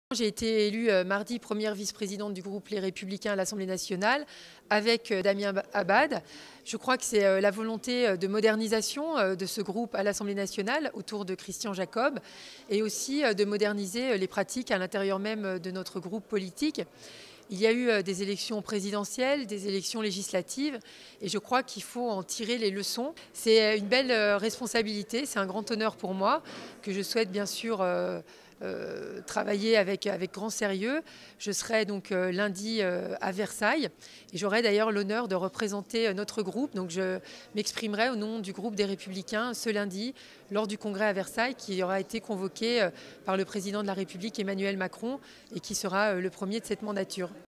Virginie Duby Muller élue Vice-Présidente du groupe Les Républicains à l'Assemblée Nationale
On parle encore de Virginie Duby Muller, qui a été nommée par Vice Présidente du groupe Les Républicains à l’AN. Une fierté pour celle qui débute un second mandat dans l’hémicycle. On l’écoute au micro de Pauline Courseaux.